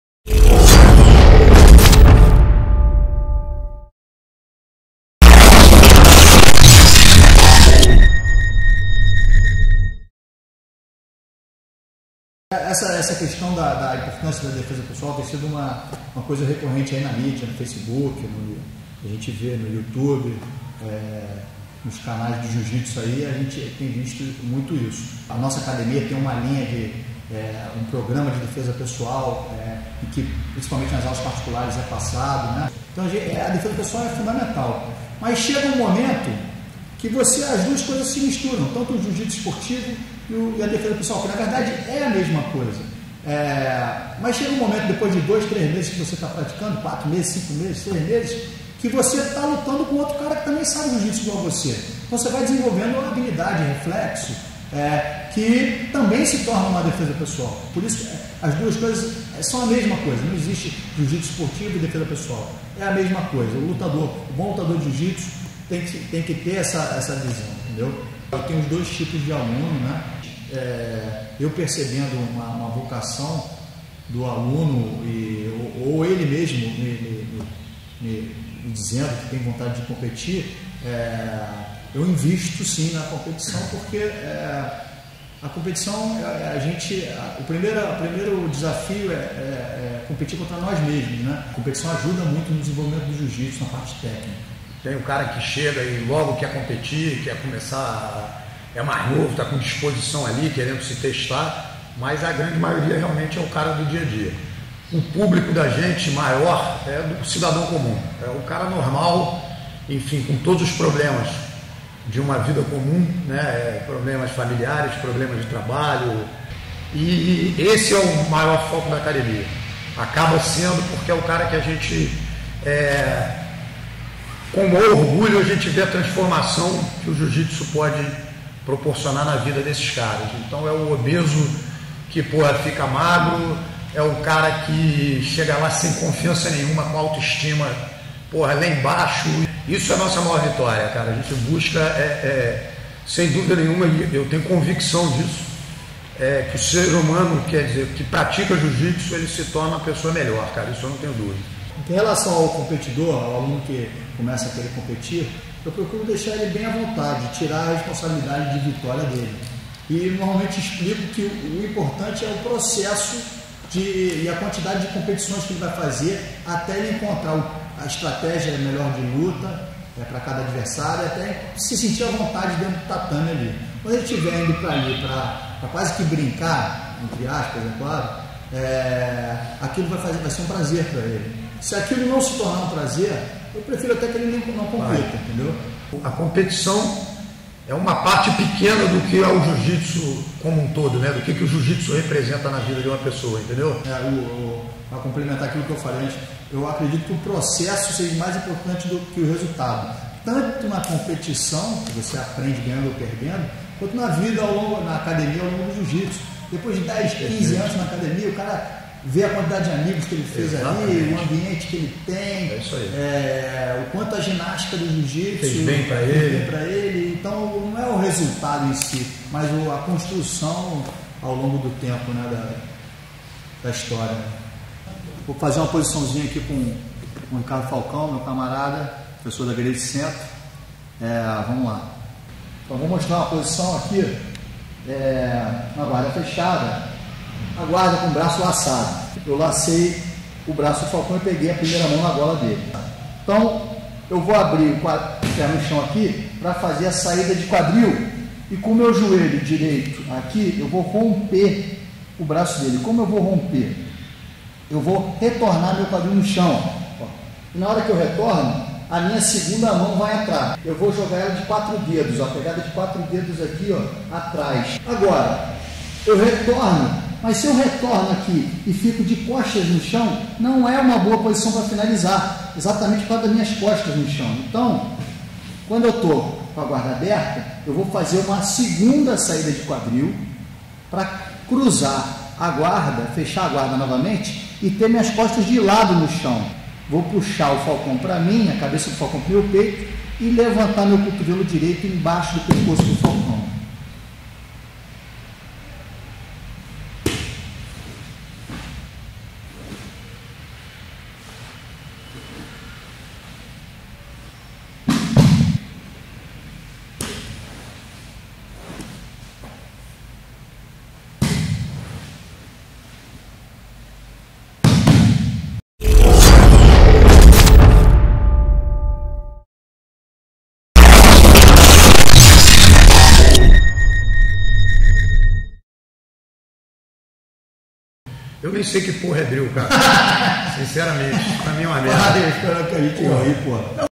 Vini Aieta on self-defense, competitive BJJ and benefits the gentle art can bring to all
Gracie Tijuca head coach Vini Aieta talks about the benefits BJJ can give normal people, and the importance of practicing it daily. He explains how BJJ can improve your technique and the importance of self-defense, and teaches us a choke from guard.

2015 ADCC champ Davi Ramos welcomed the Gallerr team and taught a leg lock starting from the de la Riva guard